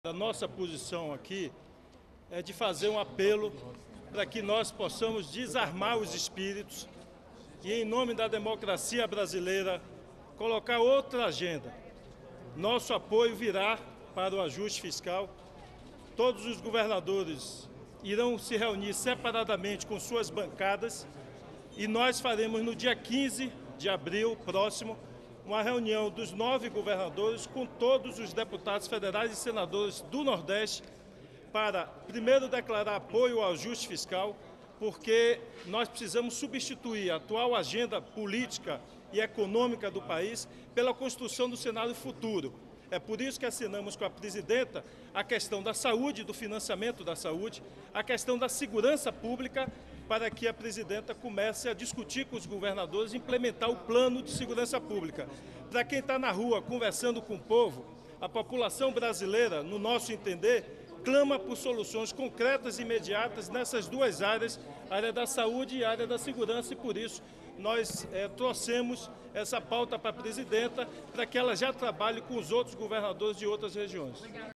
Governadores do Nordeste vão debater ajuste fiscal com deputados e senadores de seus estados
ATIVIDADES DO GOVERNO - 26.03.15: Na tarde desta quarta-feira (25),  nove governadores do Nordeste se reuniram com a presidenta Dilma Rousseff no Palácio do Planalto. Em pauta, o apoio dos governadores da região para que seja aprovado o ajuste fiscal. De acordo com o governador da Bahia, Rui Costa, os governadores se reunirão com deputados e senadores de seus estados e devem participar de uma reunião geral no próximo dia 15 de abril.